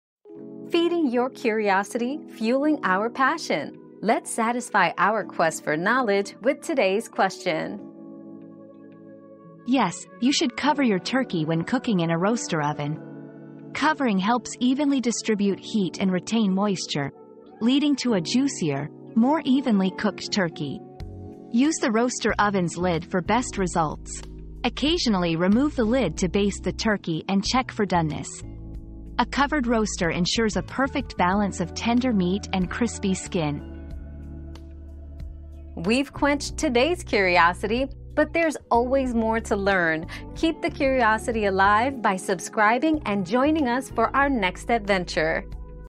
Should you cover your turkey when cooking in a roaster oven?
Covering Your Turkey in a Roaster Oven: To Cover or Not to Cover? 👉 Covering Turkey Debate 👉 Discover the secrets to juicy, evenly cooked turkey by covering it in a roaster oven. Learn how to achieve the perfect balance of tender meat and crispy skin with this simple technique.

Laura S. Harris (2024, May 15.) Should you cover your turkey when cooking in a roaster oven?
    🌐 AskAbout.video